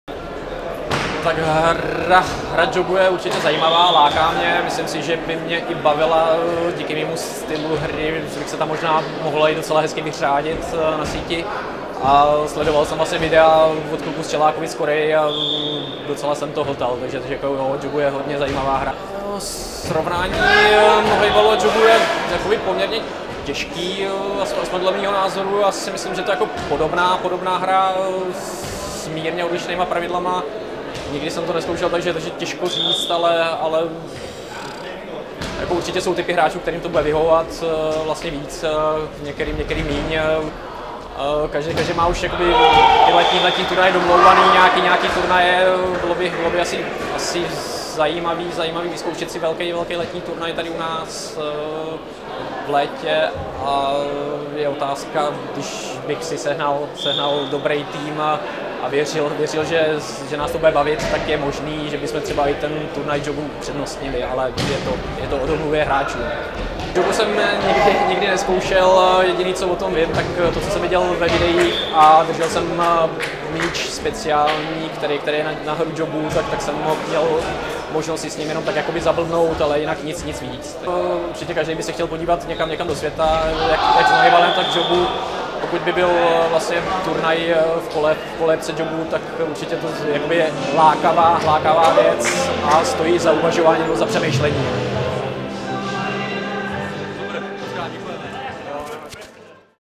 Franta Kalas vs jokgu
Na Poslední smeči se nám Franta Kalas svěřil se svým postojem k jokgu.